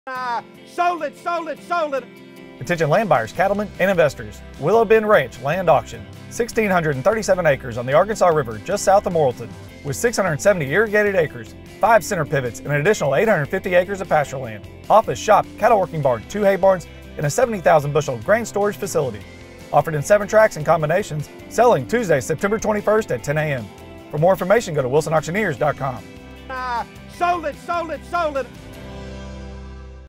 Willow Bend Ranch 1,637 Acre Cattle Farm on the Arkansas River to Be sold at Auction
Willow Bend Ranch includes 1,637+/- Tax Acres with 670+/- Irrigated & 815+/- Non-Irrigated Acres of Cropland & Pastureland Located Along the Beautiful Arkansas River in Conway County, Arkansas. The Ranch Contains 70%+/- Class 1 Silt Loam Soils & is Watered by (5) Center Pivots Offering the Potential for Conversion of Improved Pasture to Irrigated Row Crops. The Farm has a Total of (5) Active Wells.

AUCTION LOCATION: 901 E Elm St, Morrilton AR 72110
PROPERTY LOCATION: Willow Bend Road, Morrilton, AR 72110

Quick Facts:
670+/- Irrigated Acres
Center Pivots & (5) Active Wells
815+/- Non-Irrigated Acres of Cropland & Pasture
Arkansas River frontage
Office/Shop, Barns, Grain Storage

Total: 1,637+/- Acres
Irrigated Acres: 670+/- Acres

Leases and Permits:
1,485.34+/- Acres are Leased to the Current Tenant, Expiring December 31st, 2021.

Improvements:
Improvements Include an Office/Shop, Cattle Working Barn, (2) Hay Barns, 70,000 Bushels of Grain Storage, a Feedlot & Veterinarian Building.

Recreational Considerations:
Arkansas & Specifically the Arkansas River Basin are Well Known as a Prime Waterfowl Migration Flyway. The Willow Bend Ranch is a Great Hunting Location with Multiple Ponds & Substantial Arkansas River Frontage. Whitetail Deer Frequent the Ranch & Utilize the Arkansas River Basin for Travel & Cover.

Water Rights:
100% of the Seller’s Water Interest Associated with the Property will Transfer.

Mineral Rights:
100% of the Seller’s Mineral Interest Associated with the Property will Transfer.

Summary of Terms:
A 10% Earnest Money Deposit of the Total Contract Price will be Due Immediately from all Successful Bidders.
A 10% Buyer’s Premium Added to the High Bid to Determine the Total Purchase Price.
The property is being Sold “AS IS, WHERE IS” & Without any Contingencies, Including financing.
Closing will be Set 30 Days from the Auction Date or as Soon as Thereafter as Closing Documents are Available.
Broker Participation is Available. Contact our Office for Details.

Information Days:
Thursday, August 26th from 9:00am until 11:00am CST
Thursday, September 9th from 9:00am to 11:00am CST
Meet an Auction Representative at the Headquarters on Tract 3.

CLOSING COMPANY: Benchmark Title Company, LLC ~ Closing Agent: Vickie Carroll ~ 212 E. Railroad Avenue, Morrilton, Arkansas 72110 ~ ~ Fax/ 477-2339.